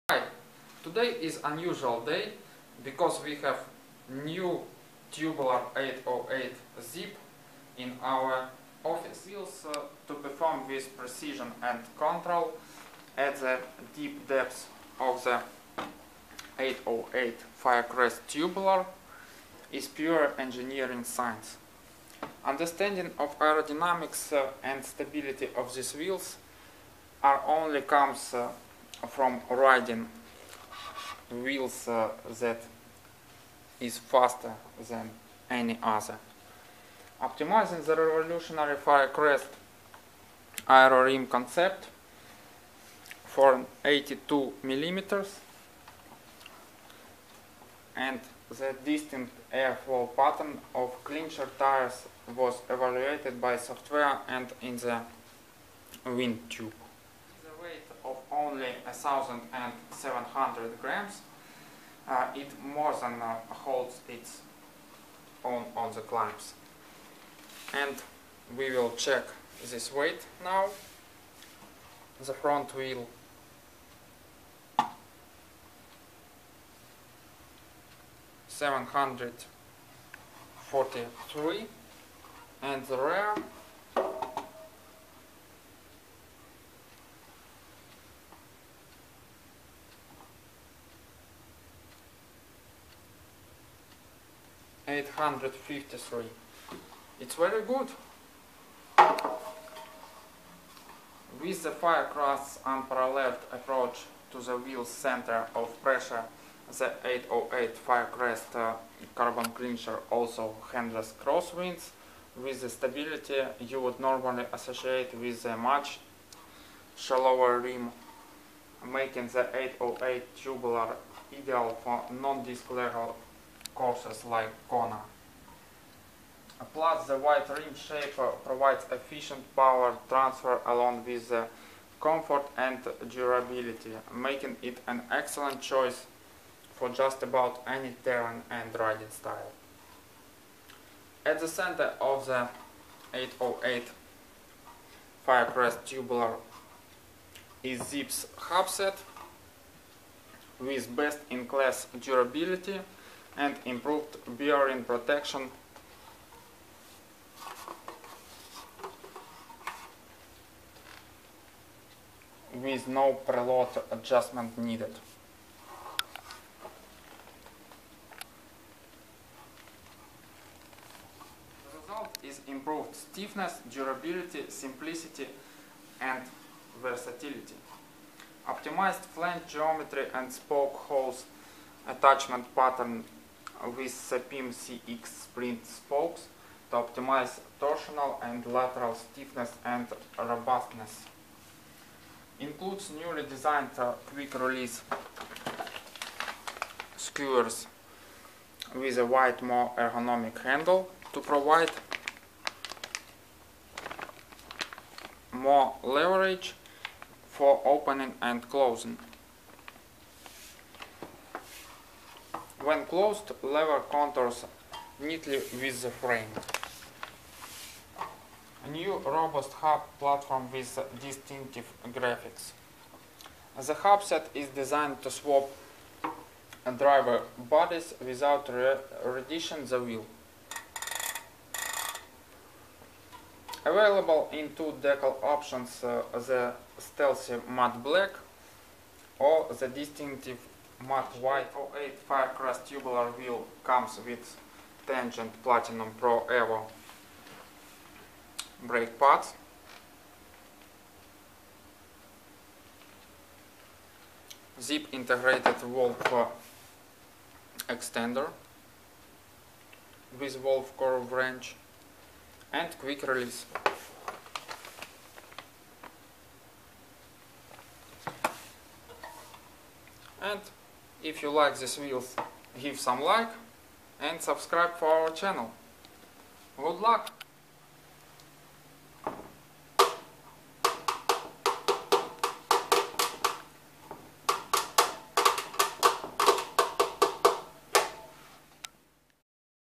The best carbon wheels - ZIPP 808 Firecrest® Tubular
Optimizing wheels to perform with precision and control at the deep depth of the 808 Firecrest® Tubular is pure engineering science. Understanding of aerodynamics and stability of this wheels are only comes from riding the wheel that, as a set, is faster than any other.
Optimizing the revolutionary Firecrest aero-rim concept for an 82mm depth and the distinct airflow pattern of clincher tires was evaluated by software and in the wind tunnel. With a weight of only 1,700 grams, it more than holds its own on the climbs.
Optimized flange geometry and spoke hole attachment pattern with Sapim® CX Sprint spokes to optimize torsional and lateral stiffness and robustness.
• Includes newly designed quick-release skewers with a wider more ergonomic handle to provide more leverage for opening and closing. When closed, lever contours neatly with the frame.
• New robust hub platform with distinctive graphics.
• The hubset is designed to swap driver bodies without re-dishing the wheel.
Each 808 Firecrest Tubular wheel comes with Tangente Platinum Pro Evo brake pads, Zipp Integrated Valve Extender with valve-core wrench, and quick release.